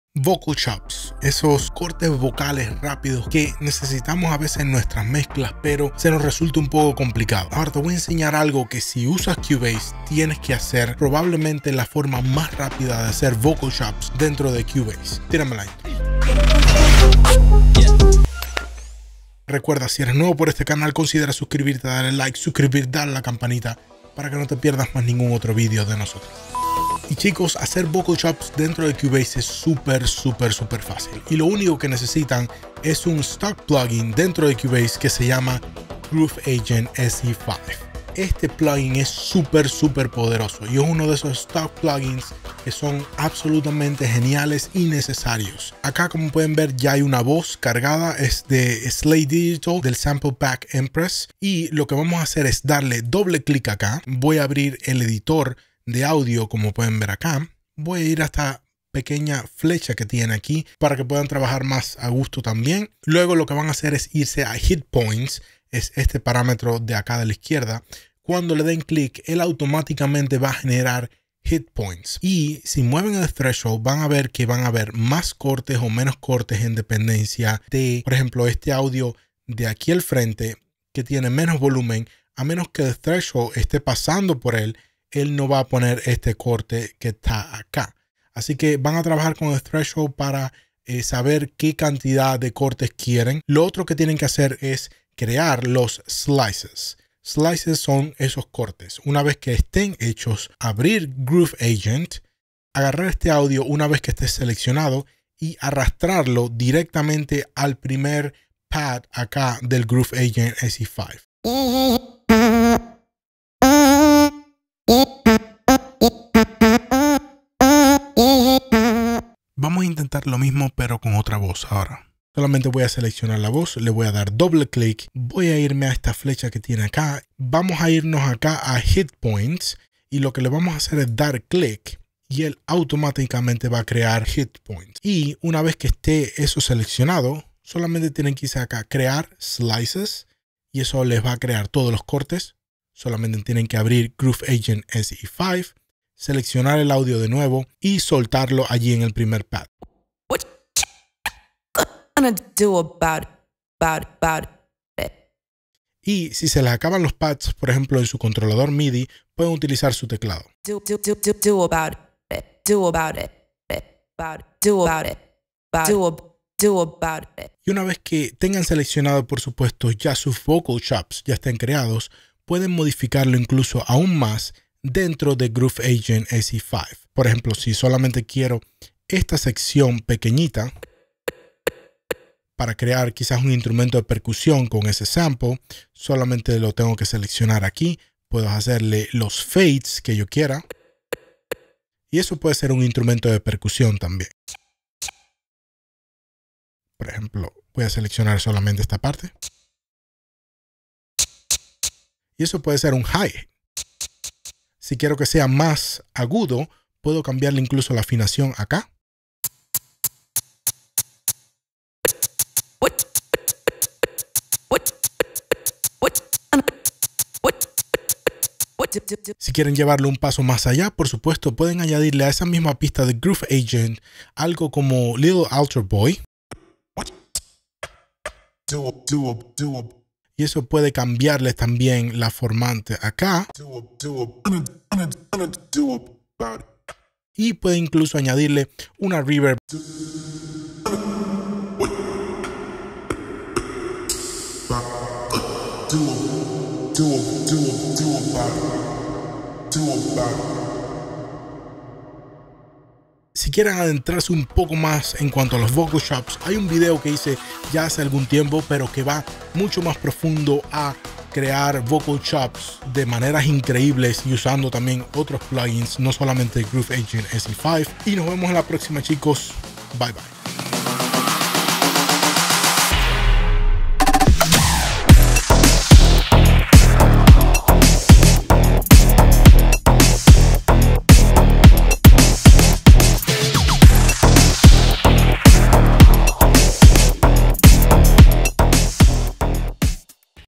Cómo hacer Vocal Chops usando Groove Agent 5 en Cubase
Cómo hacer Vocal Chops usando Groove Agent 5 en Cubase fácil, rápido, y gratis con stock plugins.

0:00 Intro
0:19 Subscríbete
0:27 Groove Agent SE 5
0:51 Creating Hitpoints & Slicing Audio
1:43 Añadir a Groove Agent
2:00 Otro ejercicio
2:47 Editando dentro de Groove Agent
3:39 Añadiendo Efectos
4:13 Outro

🎙️ EQUIPOS DE STUDIO

🎥  PARA EDICION DE VIDEO

🔴  PARA YOUTUBE

👋  SOCIAL MEDIA  👋